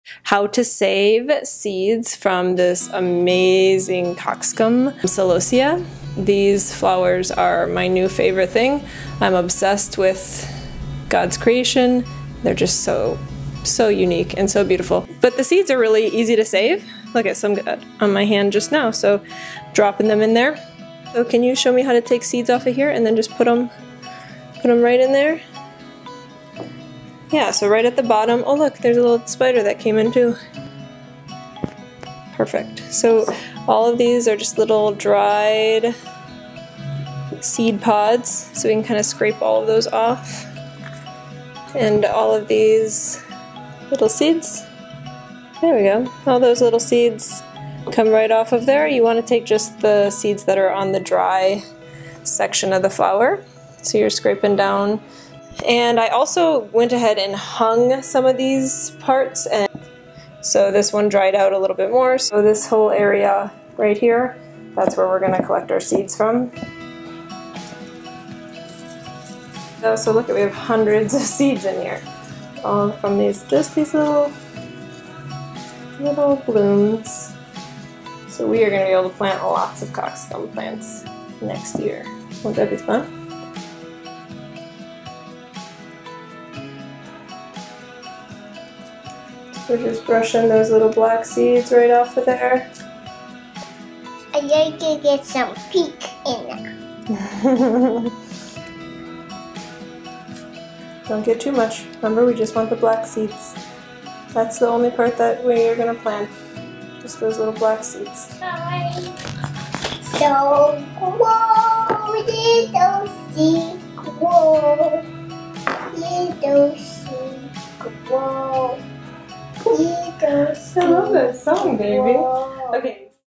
How to Save Cockscomb Celosia Flower Seeds: SUPER EASY
Cockscomb Celosia Flower is one of my new favorites! And while it is not a perennial, it is SUPER easy to save seeds and plant it year after year! Here's to putting more beauty and color into your spaces!